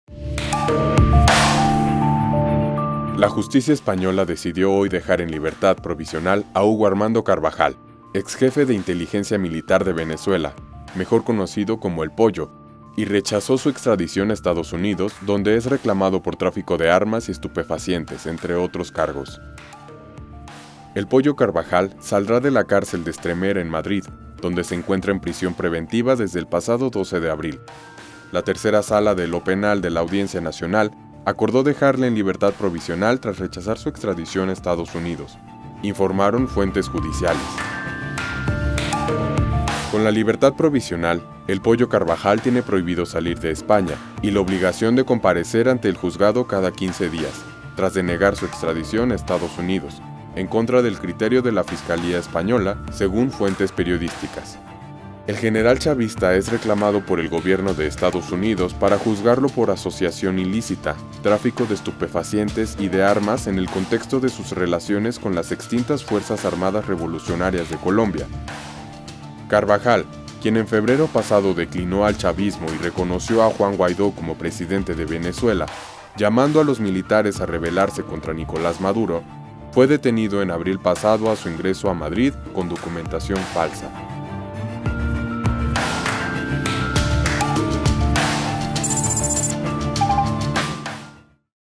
España rechaza extradición de exjefe de inteligencia de Hugo Chávez
La Justicia española decidió hoy dejar en libertad provisional a Hugo Armando Carvajal, exjefe de Inteligencia Militar de Venezuela, mejor conocido como “El Pollo”, y rechazó su extradición a Estados Unidos, donde es reclamado por tráfico de armas y estupefacientes, entre otros cargos.